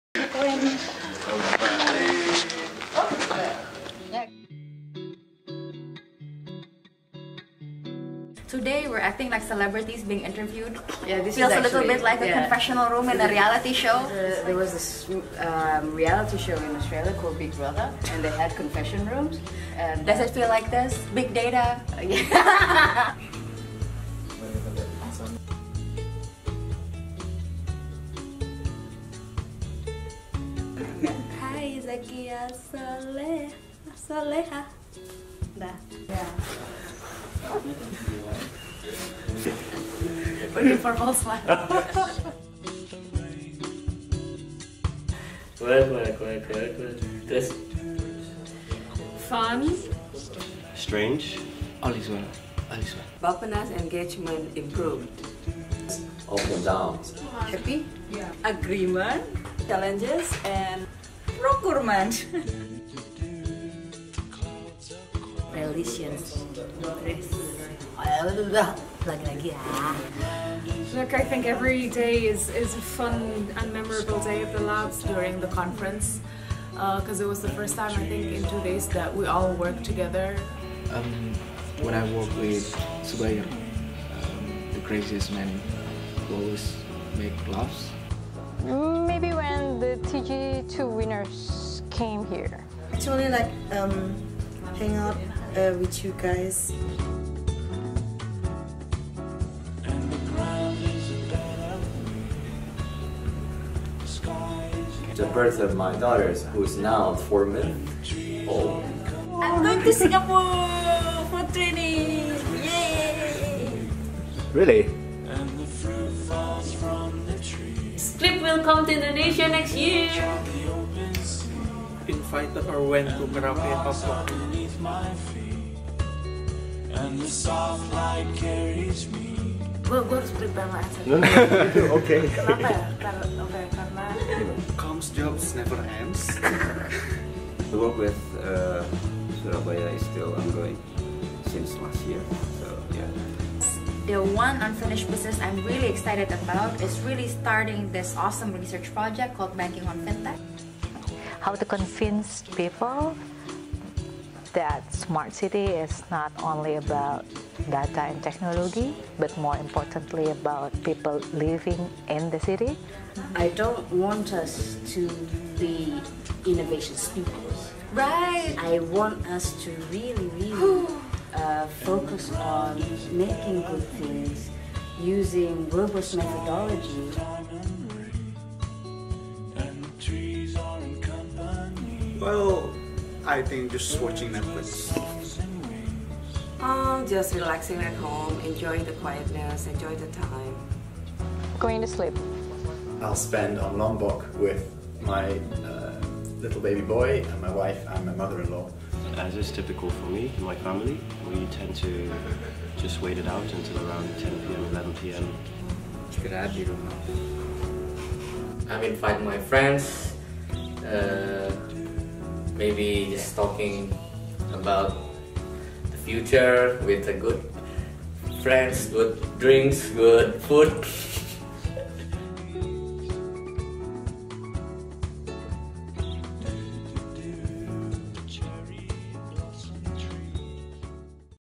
Pulse Lab Jakarta Reflection - Looking Back on 2017
As 2017 draws to a close, we at Pulse Lab Jakarta began to reflect on some of our success stories from the year. In much the style of a reality TV show confessional room, our team members sat down to reflect and share their honest-to-goodness and hilarious throwback moments, “unfinished business” and holiday plans. The video below is a compilation of their reflections and stories.